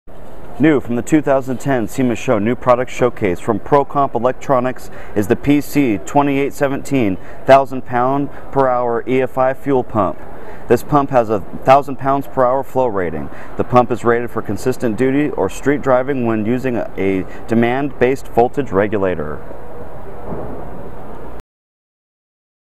PC-2817 1000lb./hr. EFI Fuel Pump from Procomp Electronics ID10920
This pump has a 1000lbs/hr. flow rating.  This pump is rated for constant duty/ street driving when using a demand based voltage regulator. for more sema videos.